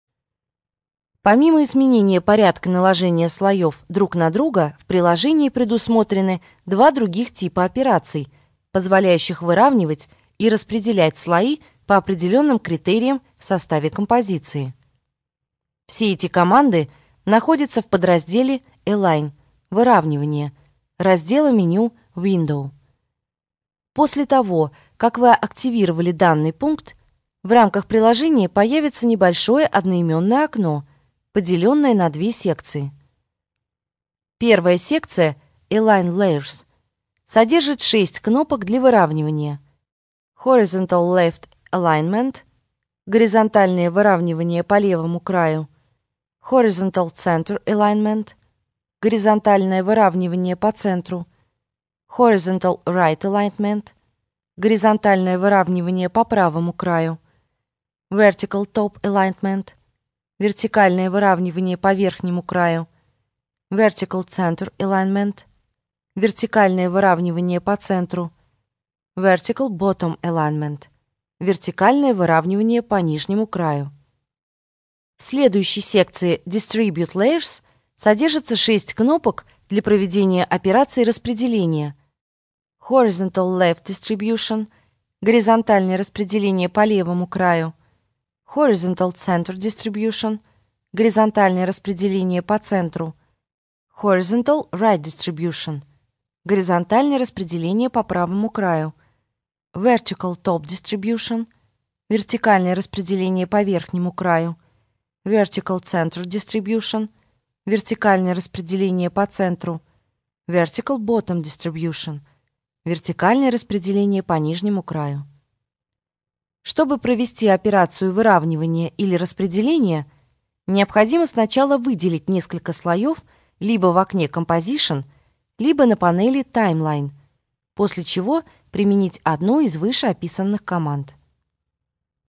Выравнивание и распределение слоев в Adobe After Effects CS4
В данном видеоуроке мы расскажем как выровнять и распределить слои по критериям в Adobe After Effects CS4.

- еще больше обучающих роликов о работе с видео.
- присоединяйтесь к нам на facebook
- здесь можно купить наши видеокурсы и смотреть видео без доступа к Интернет.

Помимо изменения порядка наложения слоев друг на друга, в приложении предусмотрены два других типа операций, позволяющих выравнивать и распределять слои по определенным критериям в составе композиций.
Все эти команды находятся в подразделе Align -- выравнивание, раздела меню Window.
После того как вы активировали данный пункт, в рамках приложения появится небольшое одноименное окно, поделенное на две секции.
Первая секция -- Align Layers, содержит шесть кнопок для выравнивания.
Horizontal left alignment -- горизонтальное выравнивание по левому краю.
Horizontal center alignment -- горизонтальное выравнивание по центру.
Horizontal right alignment -- горизонтальное выравнивание по правому краю.
Vertical top alignment -- вертикальное выравнивание по верхнему краю.
Vertical center alignment -- вертикальное выравнивание по центру.
Vertical bottom alignment - вертикальное выравнивание по нижнему краю.
В следующей секции Distribute Layers содержатся шесть кнопок для проведения операций распределения.
Horizontal left distribution -- горизонтальное распределение по левому краю.
Horizontal center distribution -- горизонтальное распределение по центру.
Horizontal right distribution -- горизонтальное распределение по правому краю.
Vertical top distribution -- вертикальное распределение по верхнему краю.
Vertical center distribution -- вертикальное распределение по центру.
Vertical bottom distribution -- вертикальное распределение по нижнему краю.
Чтобы провести операцию выравнивания или распределения, необходимо сначала выделить несколько слоев, либо в окне Composition, либо на панеле Time Line. После чего применить одну из выше описанных команд.